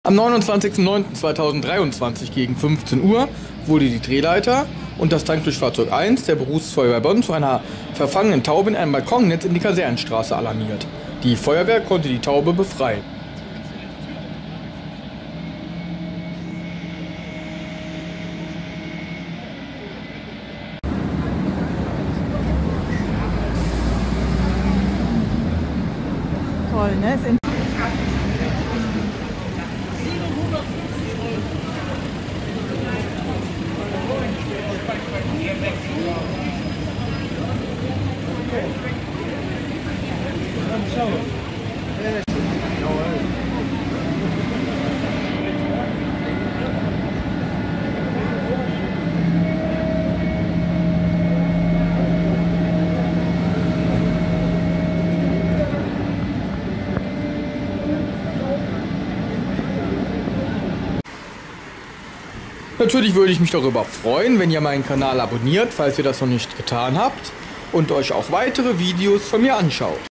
Feuerwehr befreit Taube aus Balkonnetz in Bonner Zentrum am 29.09.23
Am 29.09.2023 gegen 15:00 Uhr wurden die Drehleiter und das Tanklöschfahrzeug 1 der Berufsfeuerwehr Bonn zu einer verfangenen Taube in eine Balkonnetz in die Kasernenstraße alarmiert.
Die Feuerwehr konnte die Taube befreien.